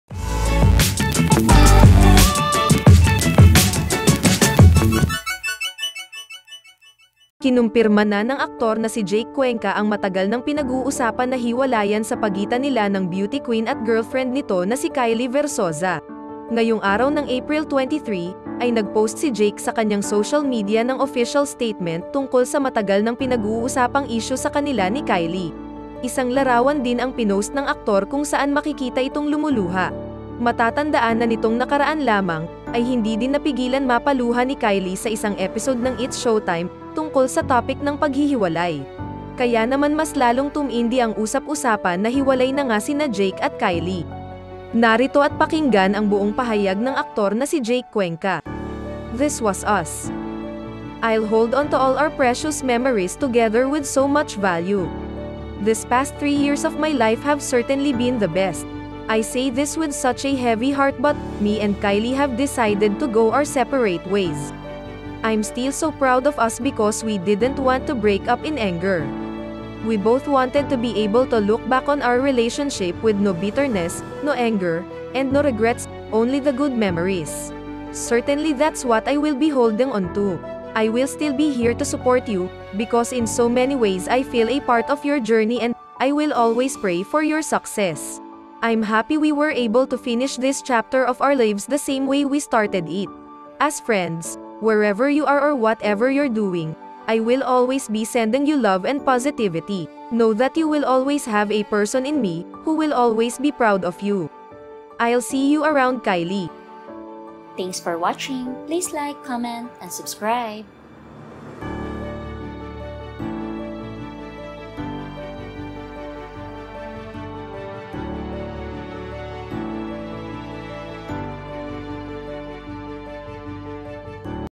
CONFIRMED! JAKE CUENCA at KYLIE VERZOSA HIWALAY NA | OFFICIAL STATEMENT
Jake Cuenca nagsalita na tungkol sa hiwalayan nila ni Kylie Verzosa. Jake Cuenca official statement.